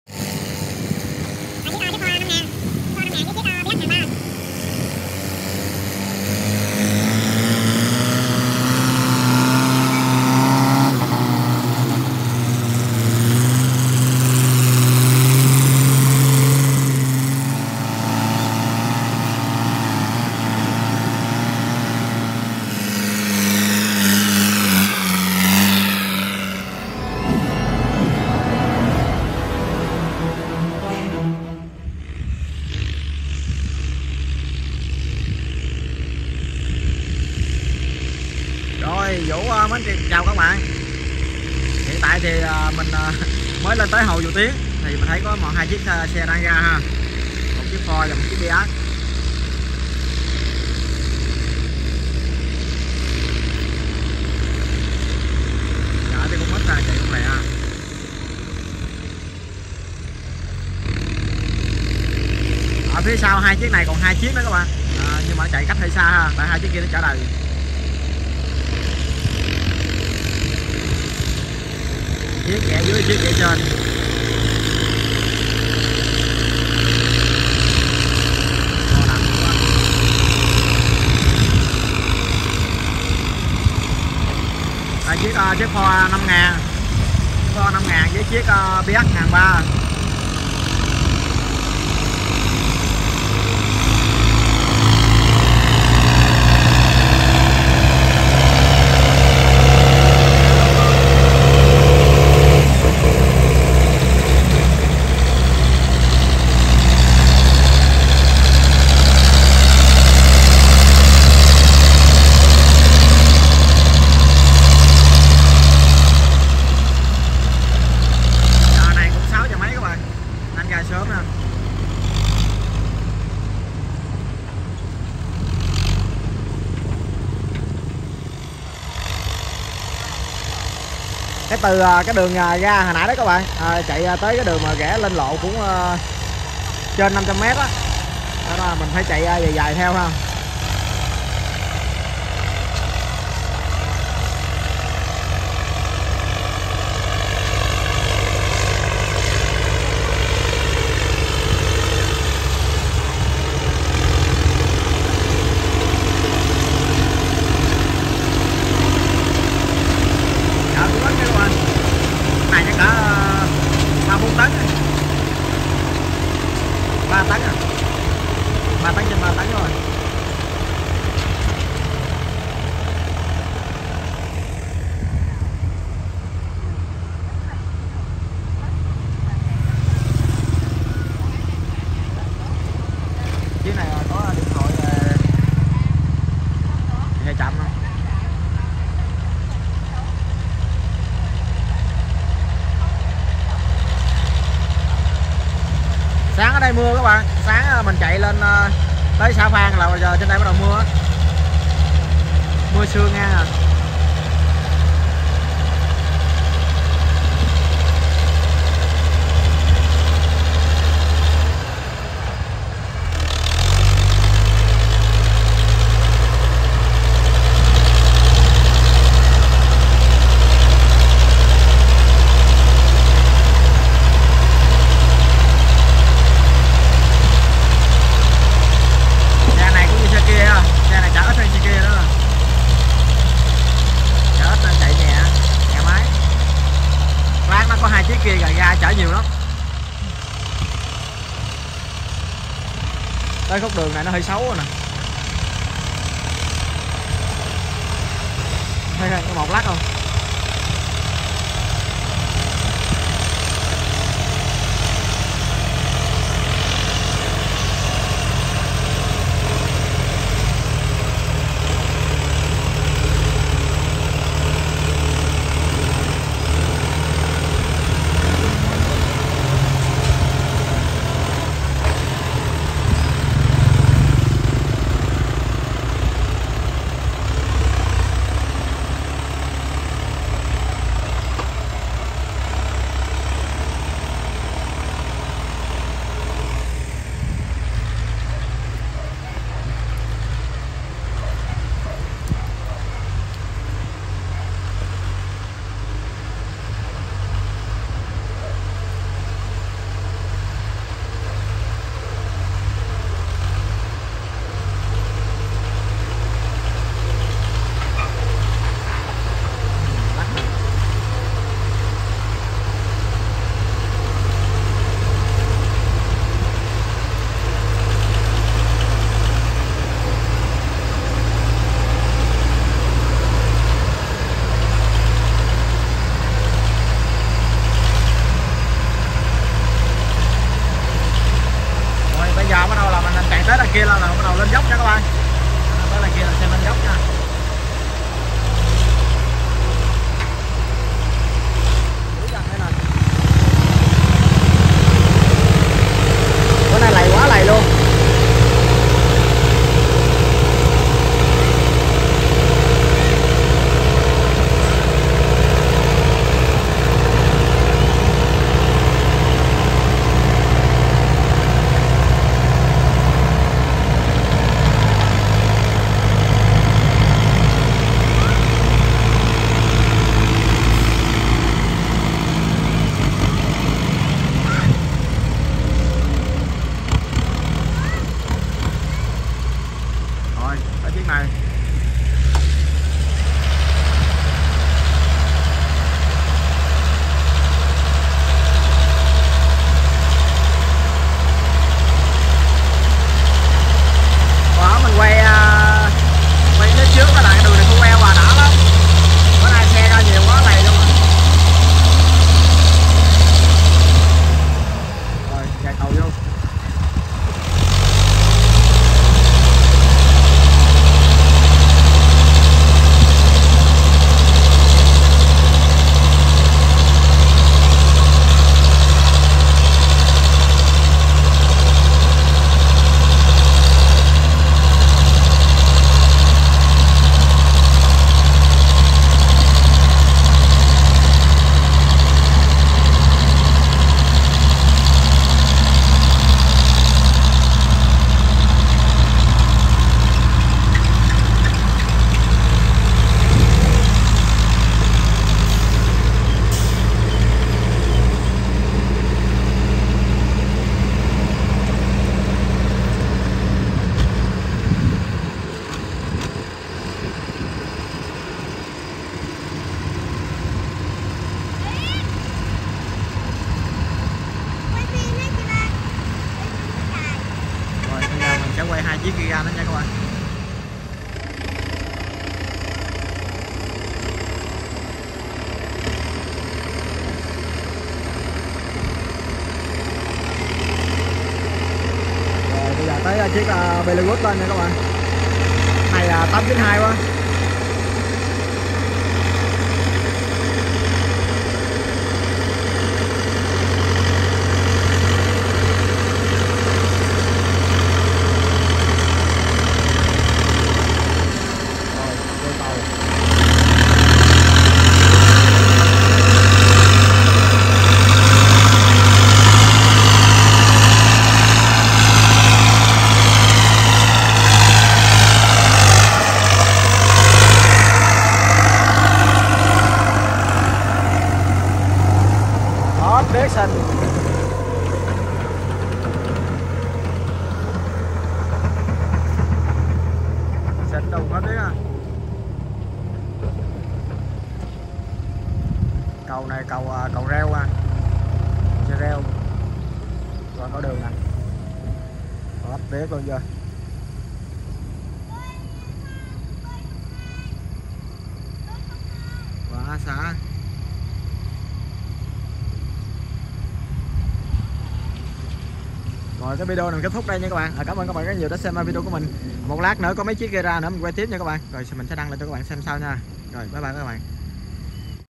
Dàn Máy Cày Khủng Fiat Ford Và Belarus Chở Mì Chạy Đường Xe ở Hồ DầuTiếng, Thiện Vũ TV
những chiếc máy cày fiat ford và belarus chở củ mì ở hồ dầu tiếng tây ninh